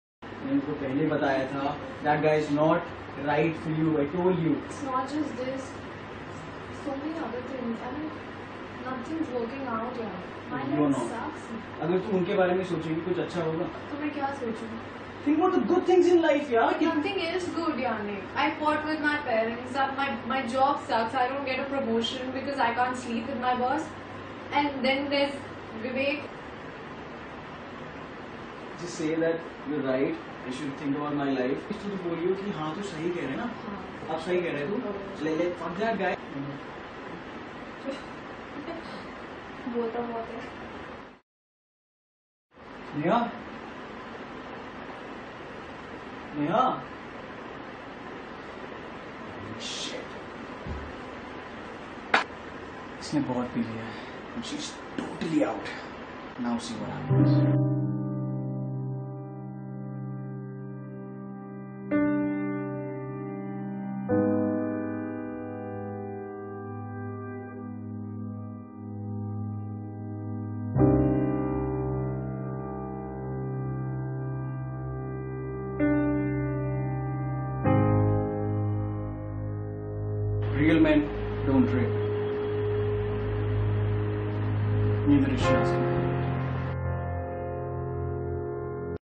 Genç Adamdan Sarhoş Sevgilisine Örnek Davranış.
"Hakiki kişiler tecavüz etmez" kampanyası kapsamında hazırlanan video, kısa sürede paylaşım sitelerinde en çok izlenen videolardan biri oldu.